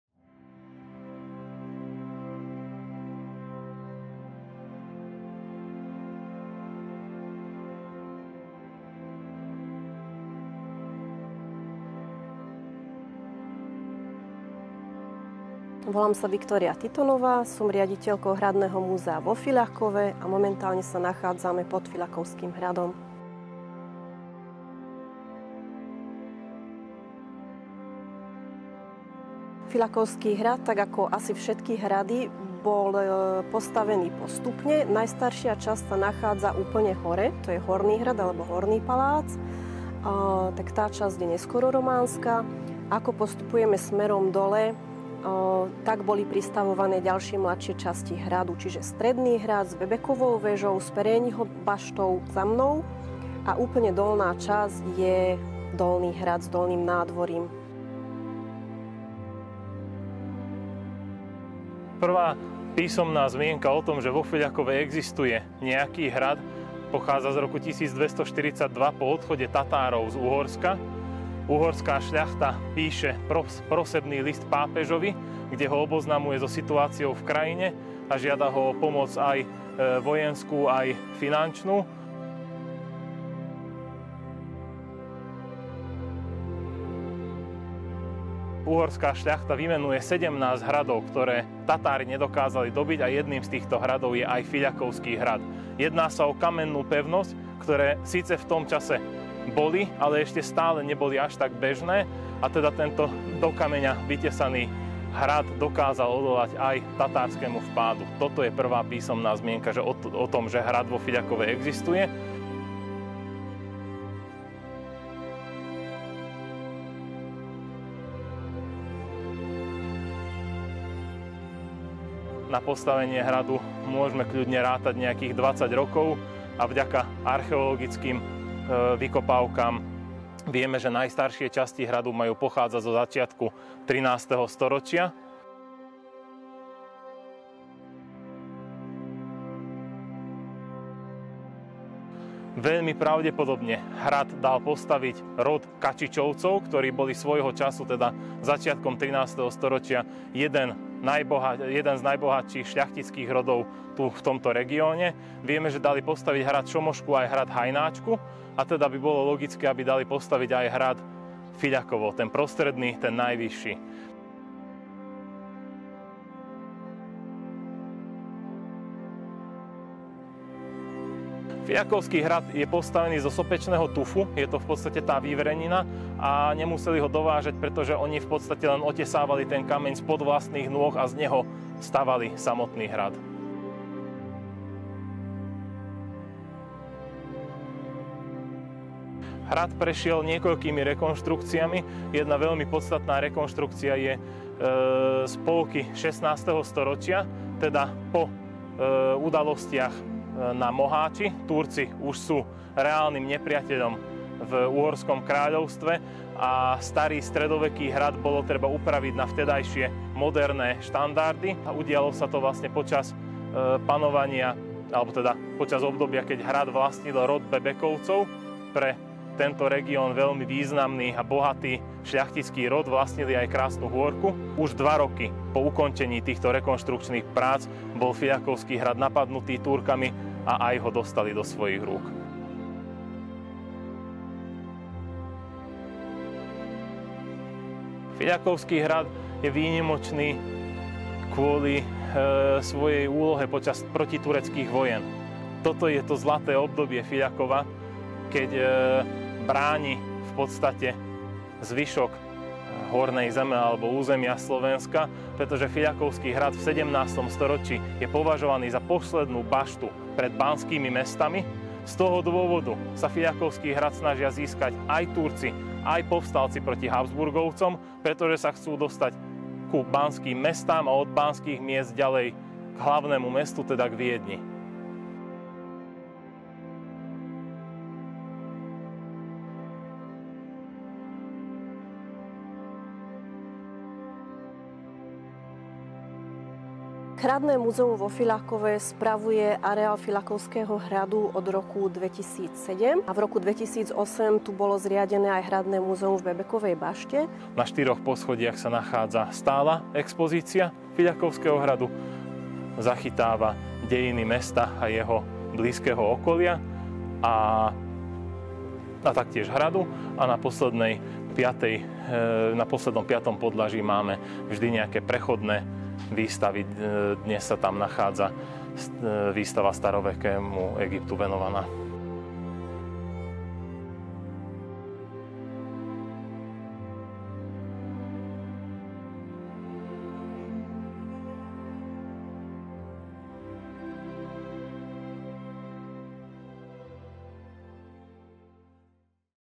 Hrad Fiľakovo
Hrad patrí k najstarším kamenným opevneniam na území dnešného Slovenska. Fiľakovský hrad bol stredoveký hrad, ktorého ruiny sa vypínajú nad mestom Fiľakovo. Prvá písomná zmienka o hrade je z roku 1242, kde sa píše, že Fiľakovský hrad odolal Tatárskym nájazdom. Hrad vlastnili viacerí majitelia, medzi nimi aj Matúš Čák Trenčiansky.

Réžia a námet: Peter Požgay
Kamera: Peter Požgay, Radovan Kúdela (dron)
Strih: Peter Požgay
Korekcia: Linda Suchá
Komentár: Patrik Kasza, Mgr. Viktória Tittonová, PhD.
Vydavateľské údaje: Národné osvetové centrum, Bratislava, 2024